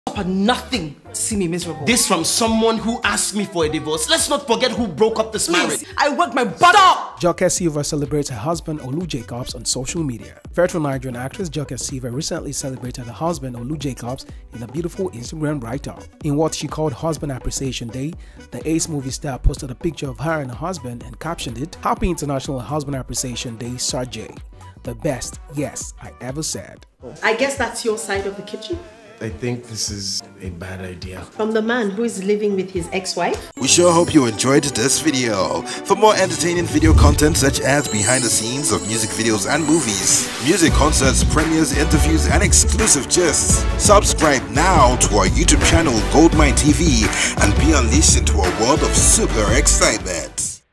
JOKE SILVA CELEBRATES HER HUSBAND OLU JACOBS ON SOCIAL MEDIA
GoldmyneTV brings you the latest events,  trending videos, movies, lifestyle and Nollywood.